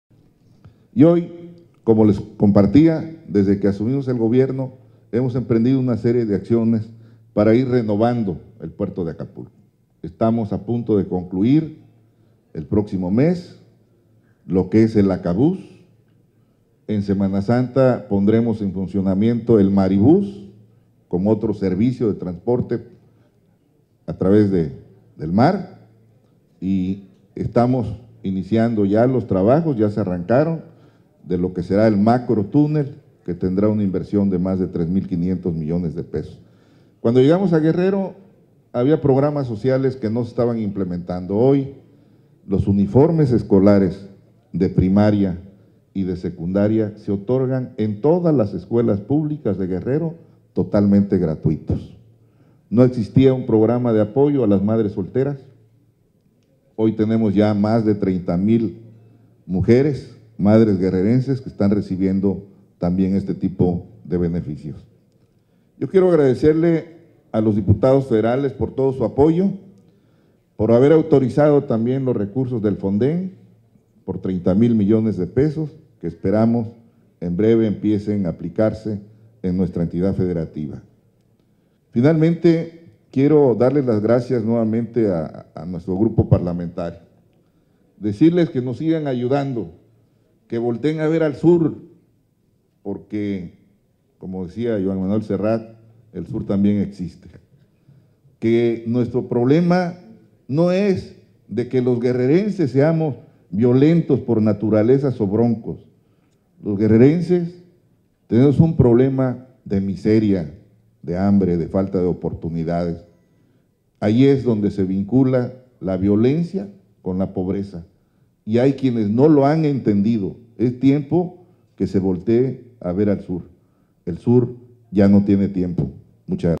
RESPALDA PRD TAREAS Y ACCIONES DE GOBIERNO DE ÁNGEL AGUIRRE
En Guerrero hay gobernabilidad, asegura el dirigente nacional del PRD, Jesús Zambrano

- Acapulco está en proceso de renovación: Aguirre Rivero
El gobernador Ángel Aguirre Rivero recibió hoy el respaldo y reconocimiento por parte de la dirigencia nacional del PRD por las tareas de gobierno realizadas, garantizando en todo momento que en Guerrero hay gobernabilidad.
Durante la sesión Plenaria del PRD de la Cámara de Diputados, realizada en Acapulco, el mandatario estatal refirió que el puerto se encuentra en proceso de renovación a través de nueva y mejor infraestructura.
Ejemplificó el sistema, moderno de transporte Acabús y el Maribús, así como el Macrotúnel.
Recordó que aunado a esta inversión que redituará en mayores empleos para los guerrerenses, se han aplicado programas sociales como la entrega gratuita de uniformes, y el apoyo para más de 30 mil madres solteras.

Respecto al actuar de los ciudadanos que quieren coadyuvar en materia de seguridad, recordó que hay un decreto para regularizarlos y conformar la denominada policía rural, donde se tienen designadas sus funciones y competencias bajo el amparo de la ley.
Por último, agradeció la solidaridad y respaldo de los senadores, diputados y dirigencia del PRD, a quienes convocó a que contribuyan para que se voltee a ver los estados del sur que tienen problemas de miseria y pobreza, factores que son vinculados a las acciones delictivas; "el sur ya no tiene tiempo, el sur ya no puede esperar", subrayó.